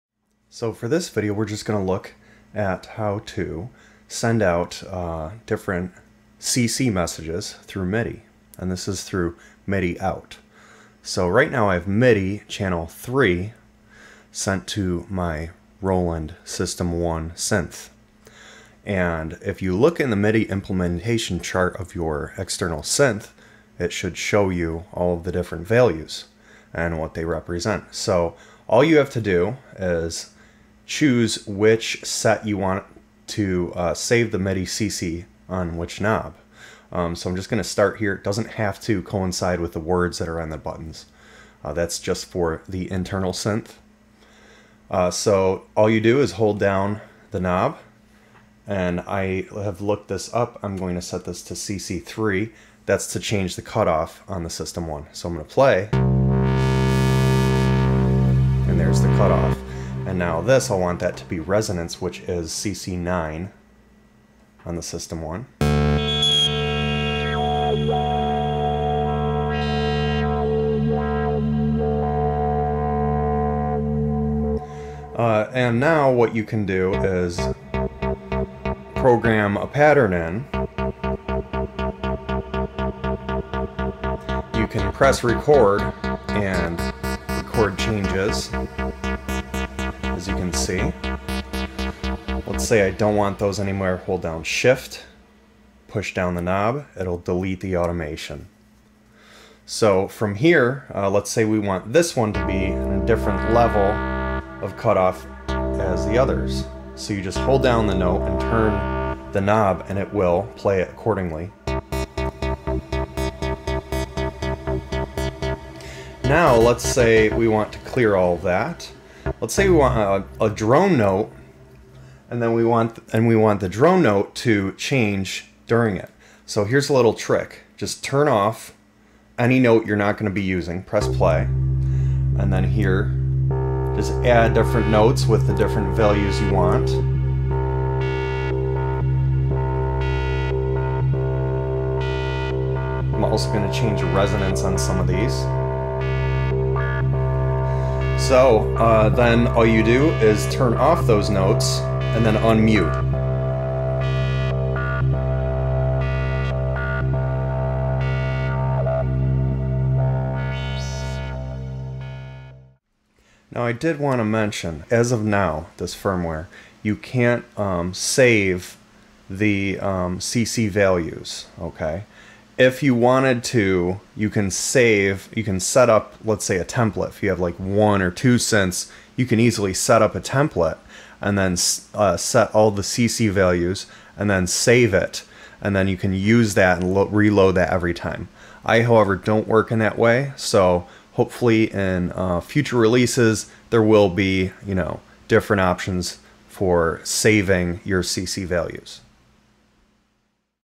How To Program Midi CC For External Synths // Synthstrom Deluge Tutorial
This tutorial is to show how to program the gold knobs on the deluge to send midi control changes to external synths. For this example we use a System 1 Promars Plug Out.